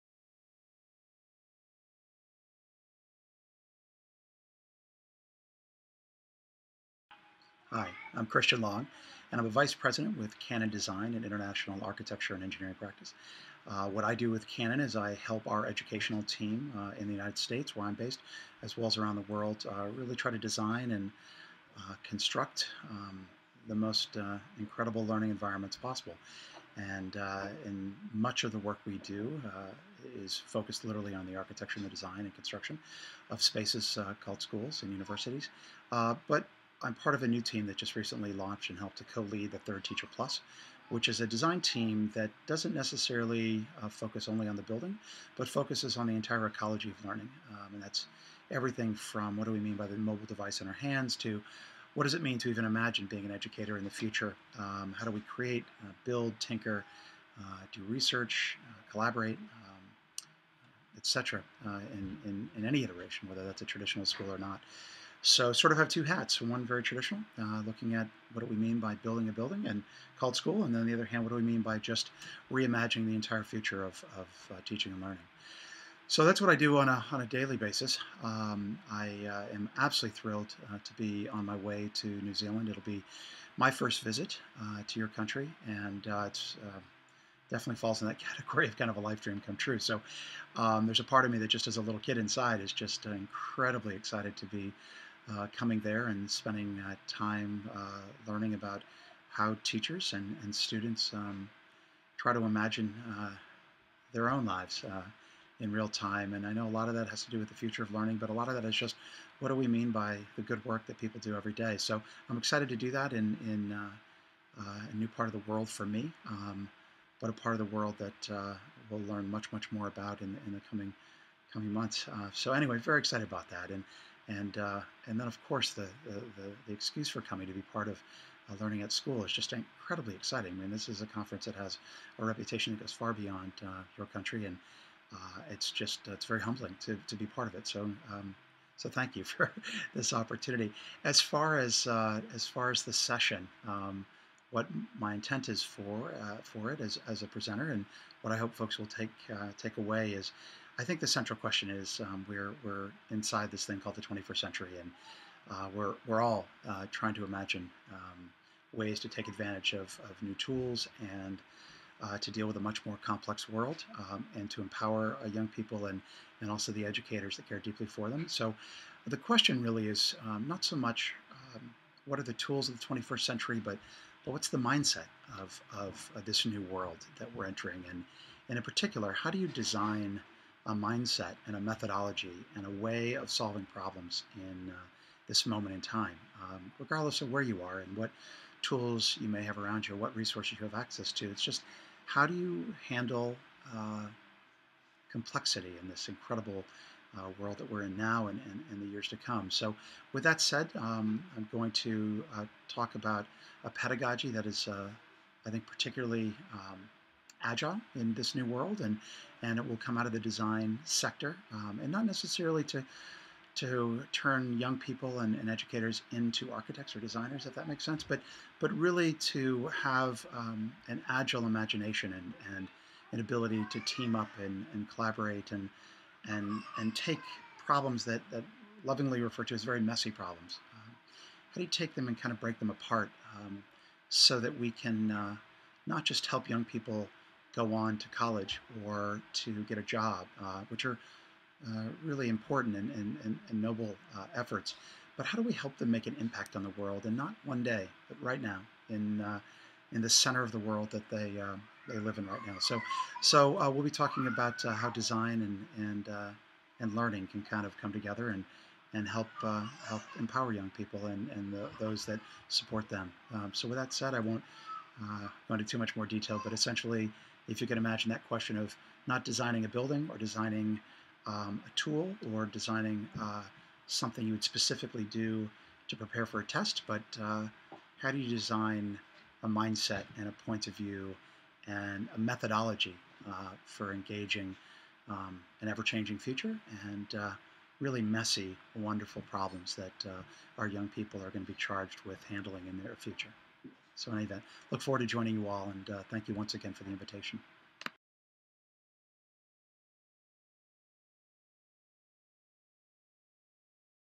Learning@School 2012 : Speakers Taster - Christian Long
Christian Long is one of our Keynote speakers and offers here an insight into what he'll be exploring at next months Learning@School 2012 conference in Hamilton.